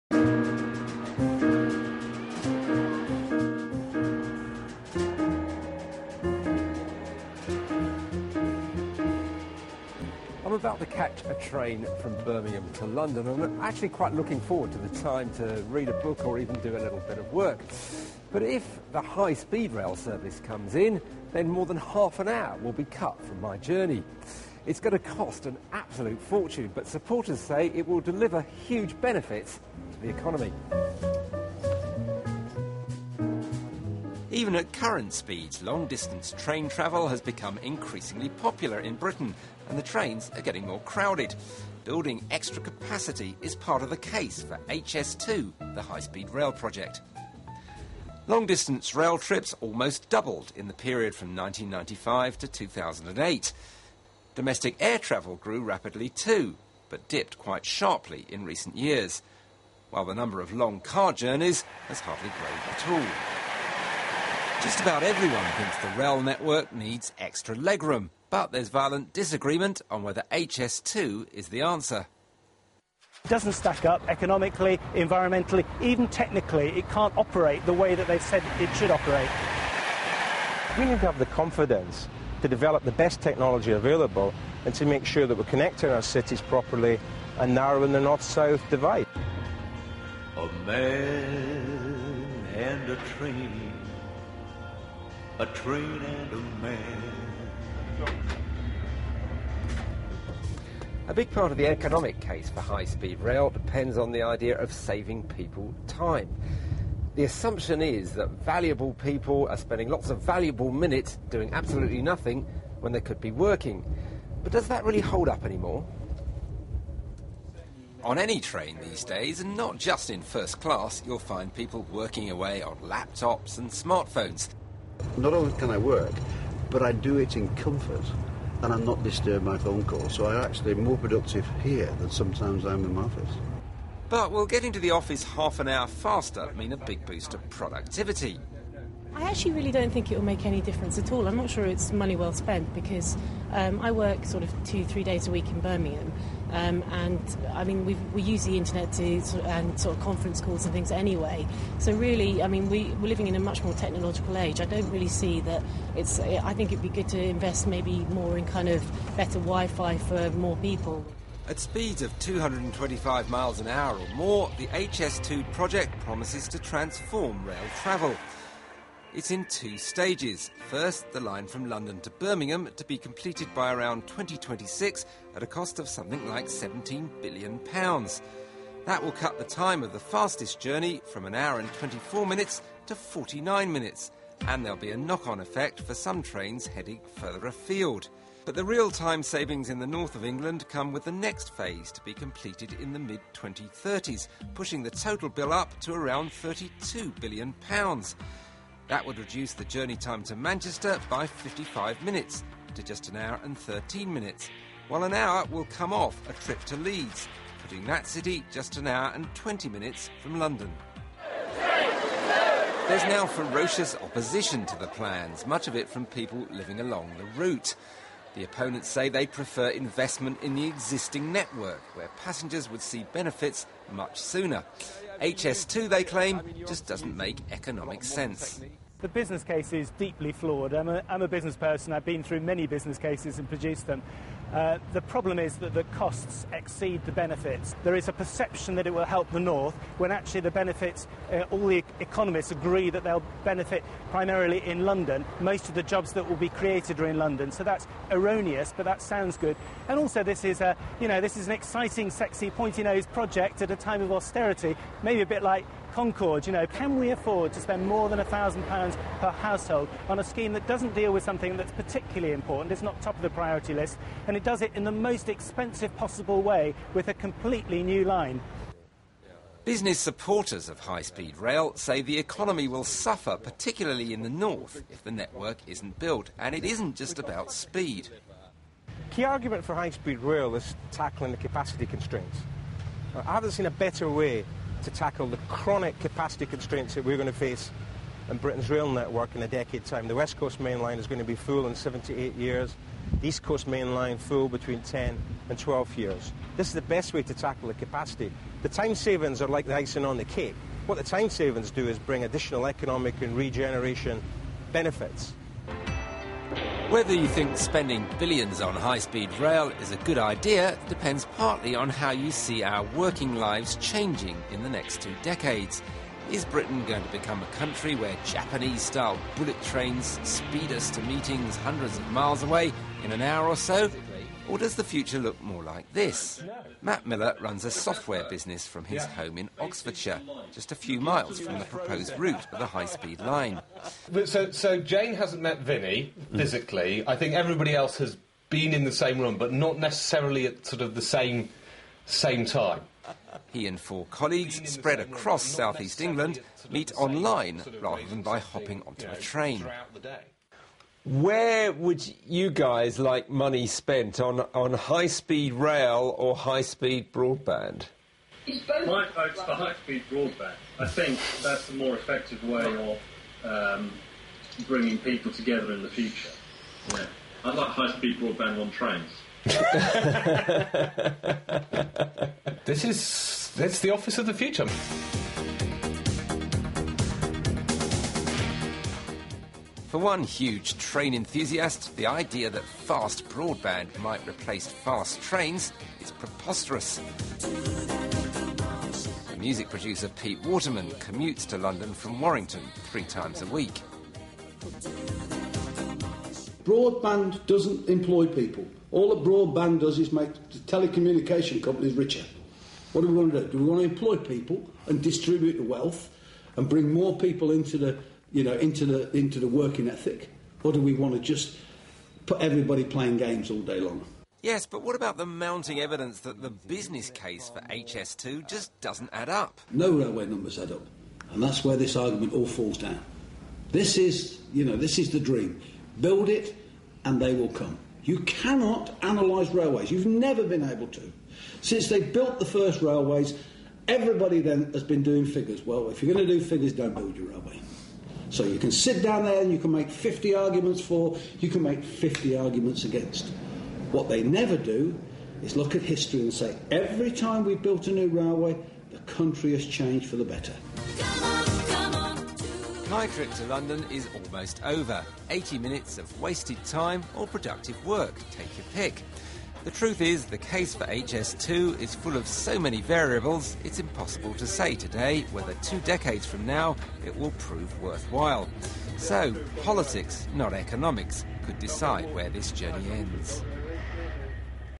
Is the UK s high speed rail project a waste of money Video BBC News
Long-distance train journeys would be cut by more than half an hour, according to advocates of Britain's high speed rail project (HS2). Trains will travel at speeds of more than 200mph but the project will cost £32bn. Will getting into the office half-an-hour faster really increase productivity? Or is the project a waste of money?.